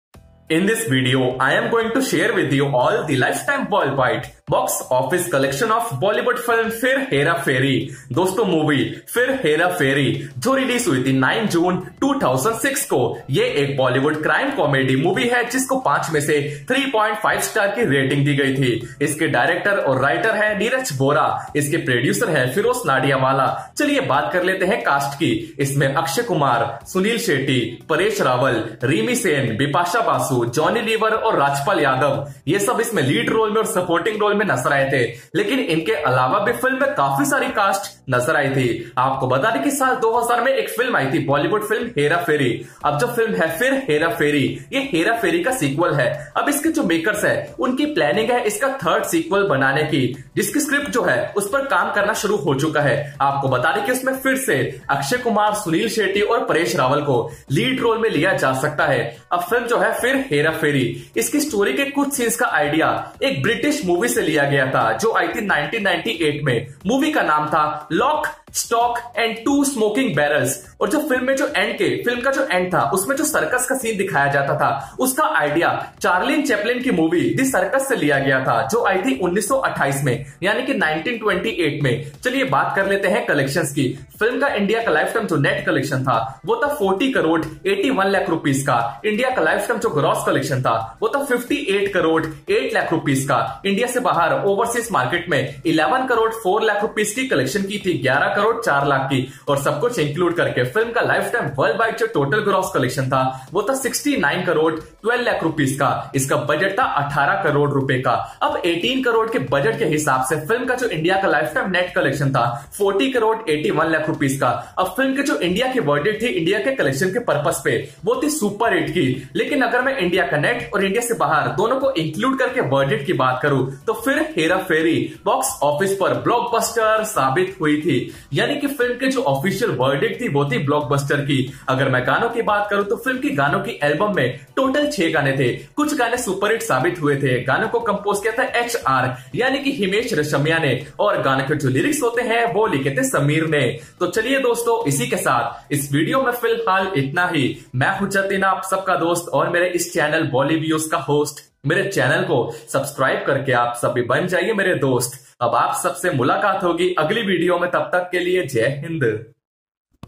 Akshay Kumar PHIR HERA PHERI 2006 Bollywood Movie LifeTime WorldWide Box Office Collection
Akshay Kumar, Suniel Shetty, Paresh Rawal, Rimi Sen, Bipasha Basu, Johnny Lever, Rajpal Yadav